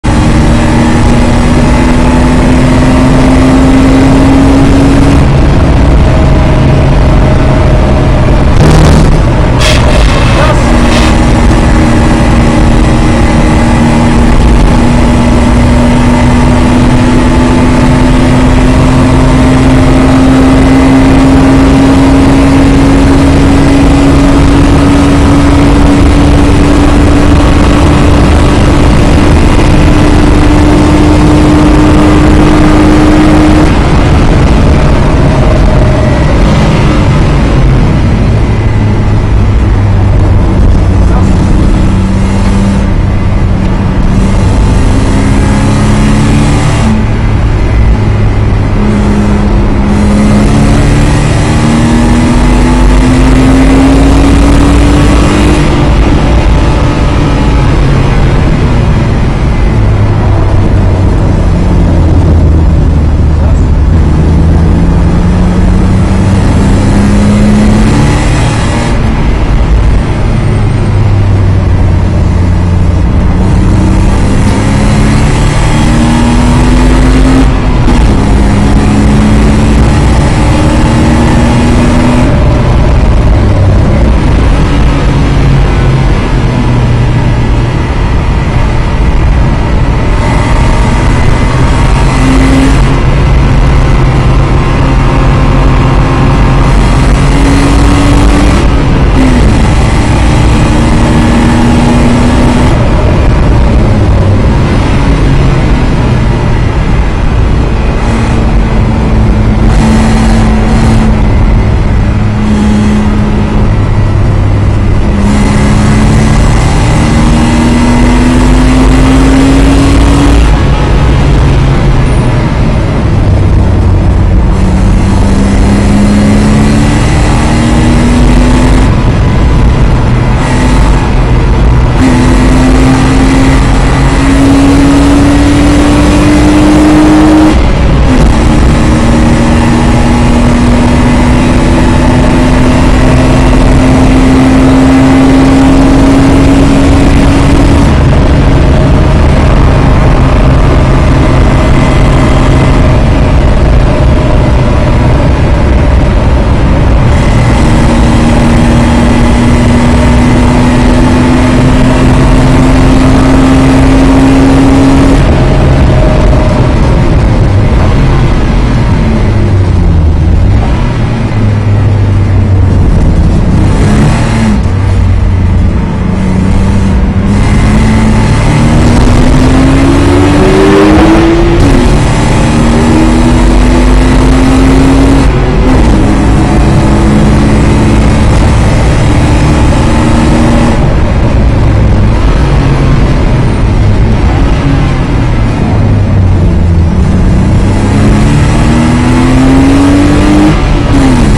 RN #1 Onboard video SPA, BMW 325, 03:26.901
LAUWRENS KARTONO in BMW 325 achieved a lap time 03:26.901 on SPA on the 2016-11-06 at 16:18.